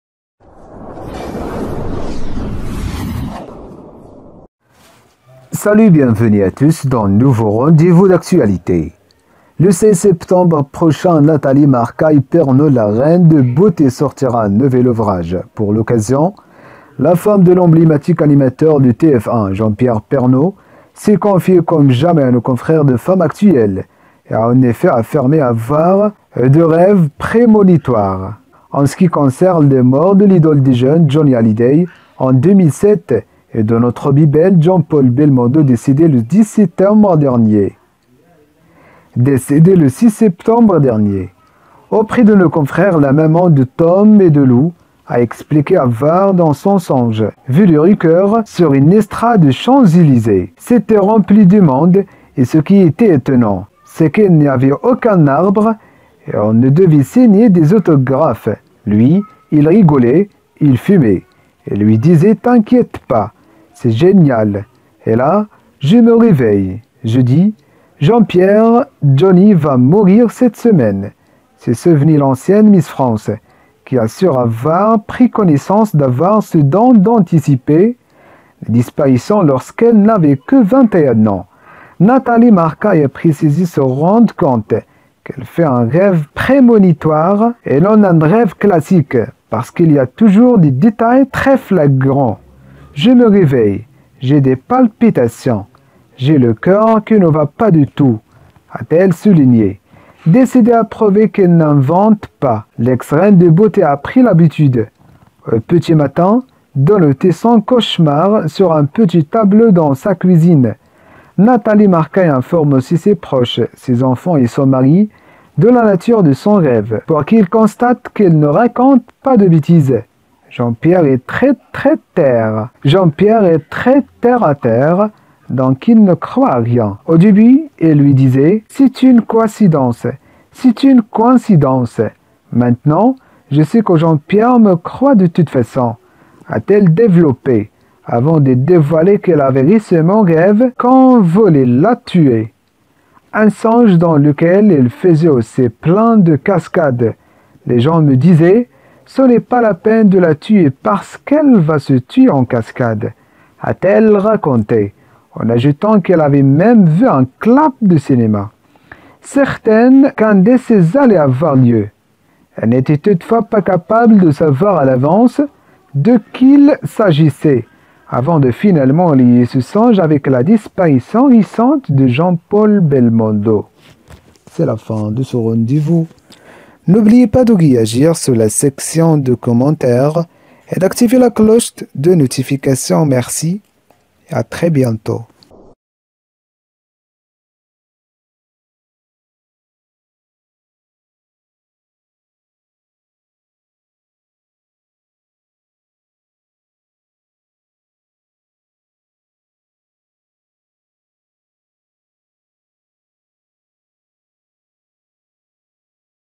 Nathalie Marquay raconte ses rêves prémonitoires, qui annoncent souvent de tristes nouvelles
Le seize septembre prochain, Nathalie Marquay Pernaut, la reine de beauté sortira un nouvel ouvrage. Pour l’occasion, la femme de l'emblématique animateur de TF1, Jean-Pierre Pernaut s’est confiée comme jamais à nos confrères de Femme Actuelle et a en effet affirmé avoir eu des rêves prémonitoires en ce qui concerne les morts de l'idole des jeunes Johnny Hallyday, en deux mille sept et de notre bebel, Jean-Paul Belmondo, décédé le six septembre dernier.